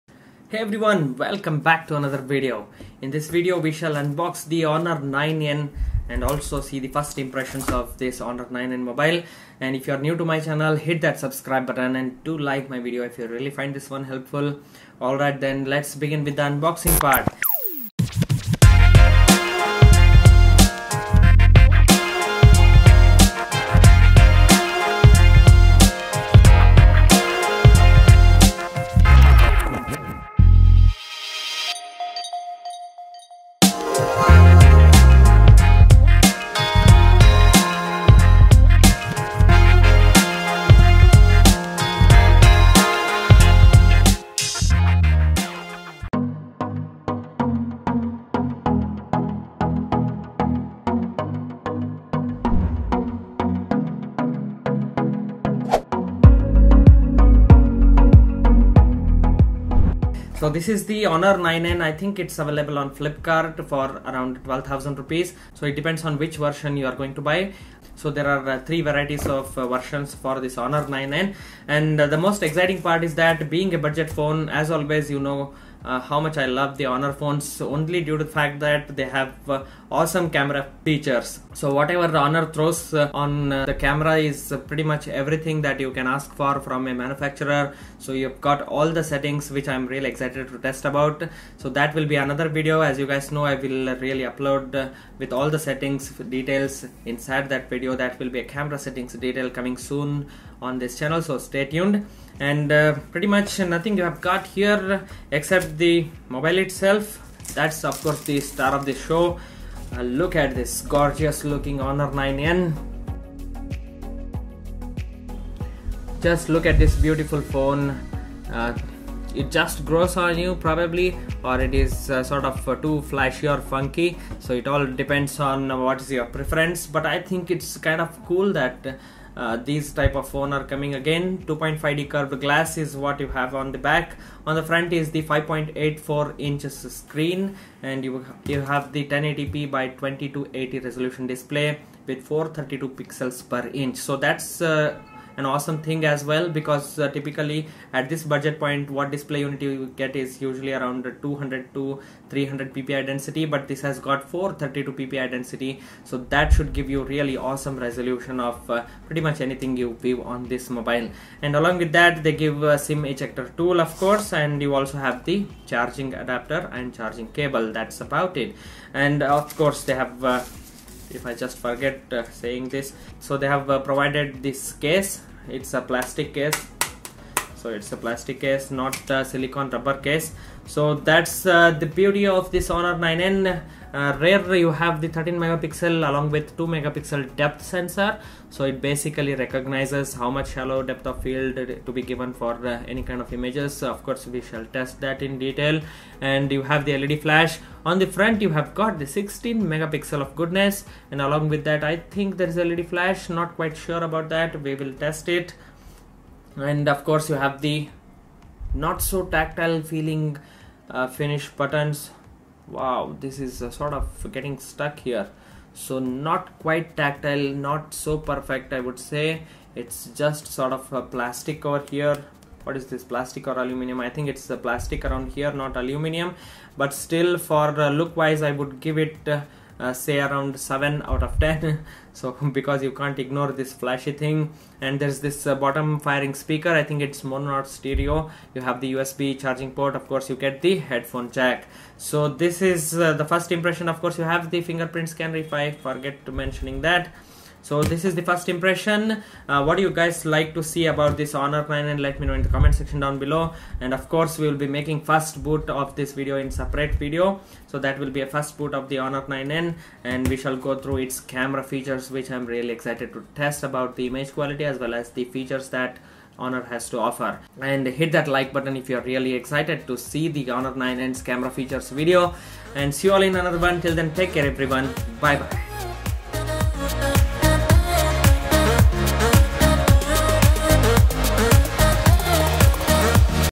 Honor 9N (9i) First Impressions | Overview & Unboxing
Back with another video. Today we will be looking at the budget beauty - The Honor 9N (also called Honor 9i in China).

Honor 9N comes with a HiSilicon Kirin 659 and a whopping ~432 ppi density at 1080 x 2280 pixels, 19:9 ratio is awesome. It also comes with a 2.5D glass at the back and it gets a finger-print scanner, all for just starting at Rs.11,999 for a 3GB | 32GB version.
~ 13MP + 2MP for depth sensor with LED flast at the Back
~ 16MP Front Cam
~ 1080p at 30fps
It comes loaded with pro features for camera and that's what we will be looking in the next video!

Have you hopped onto my facebook and instagram page ?